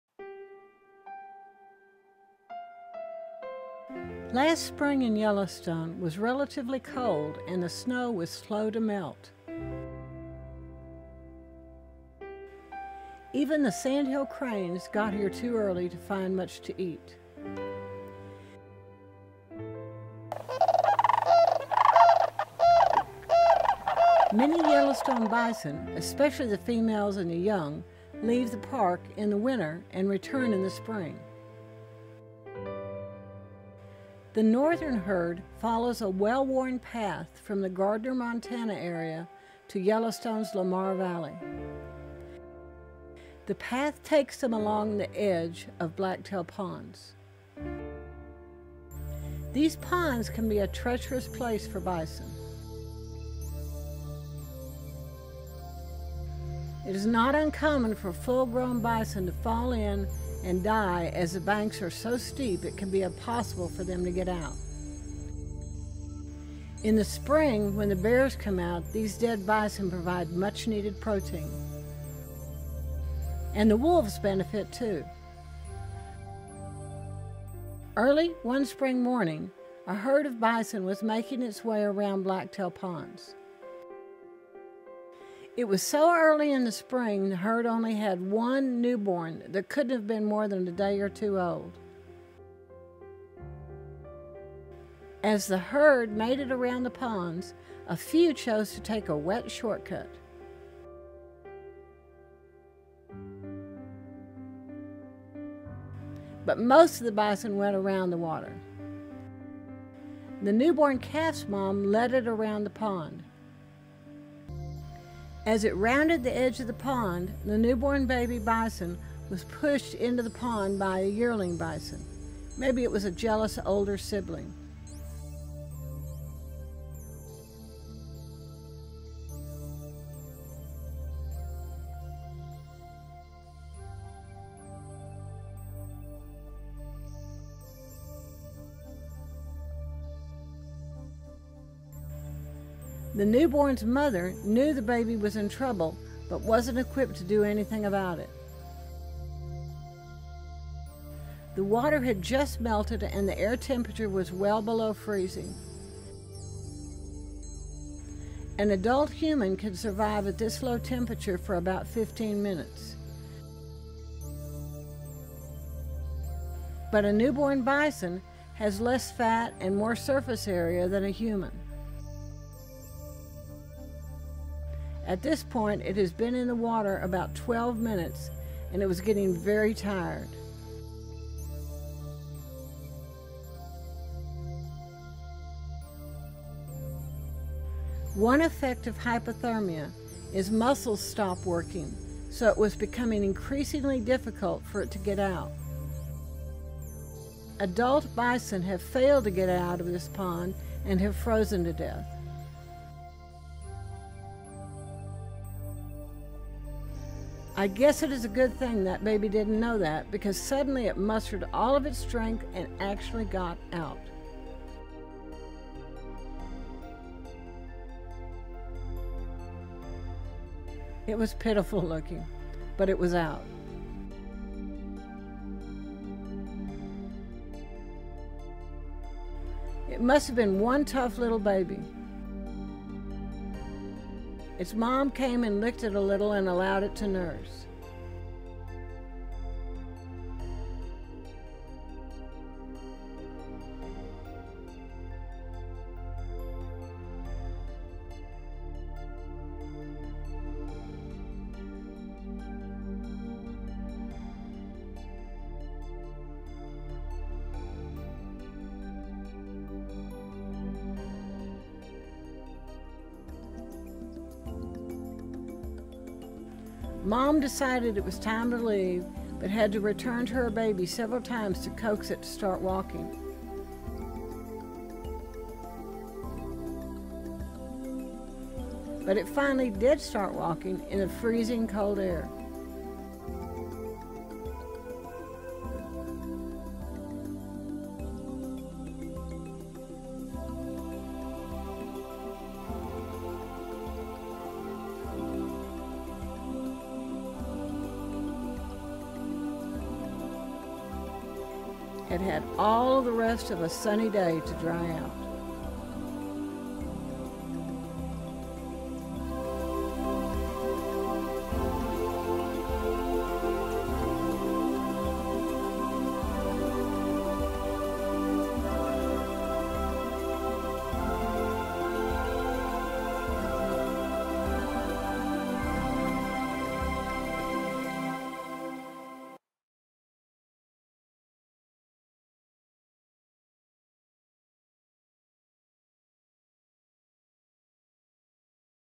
Baby Bison Falls into Blacktail Ponds in Yellowstone
As a herd of bison was moving back into Yellowstone after the winter a newborn bison fell into one of Blacktail Ponds, where bison die every year.